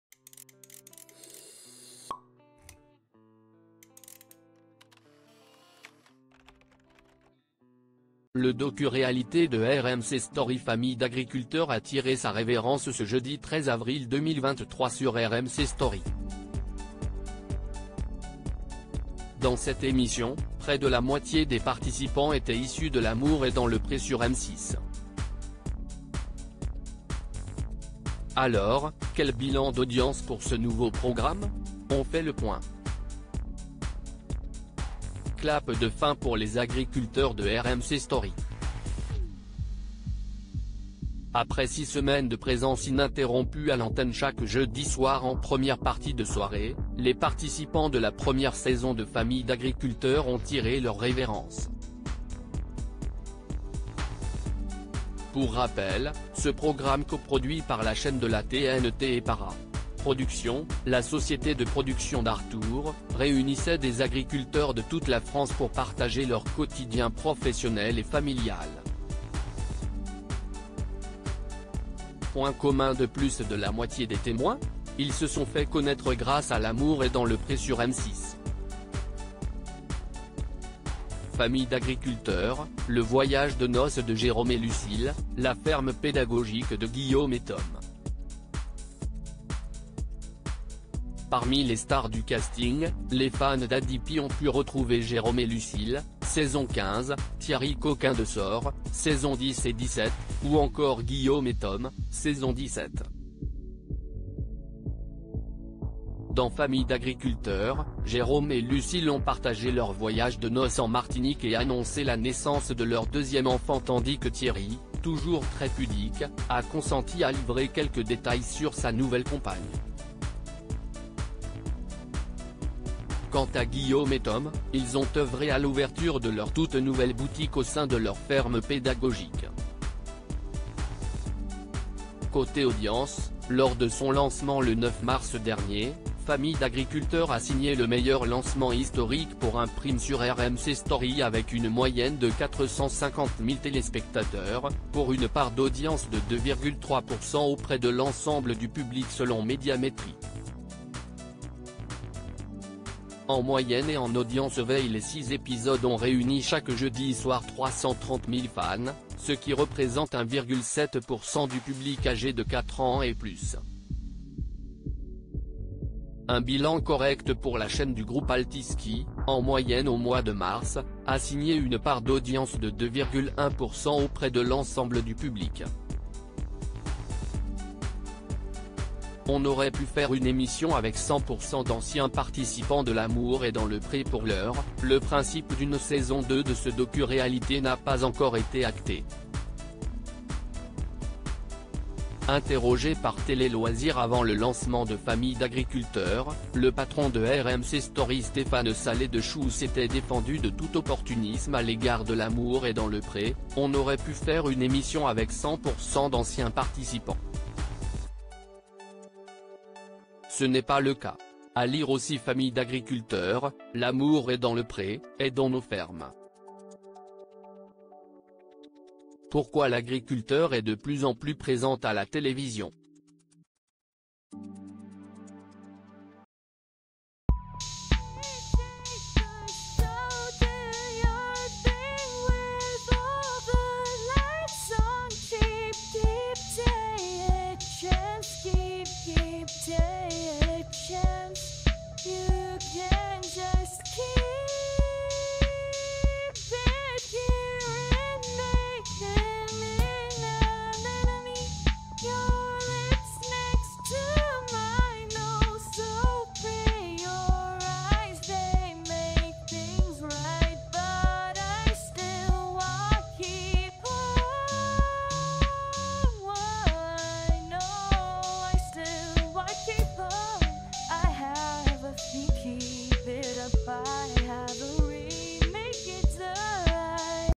Familles d'agriculteurs : les anciens de L'amour est dans le pré ont-ils cartonné sur RMC Story ? On
Familles d'agriculteurs : les anciens de L'amour est dans le pré ont-ils cartonné sur RMC Story ? On fait le bilan des audiences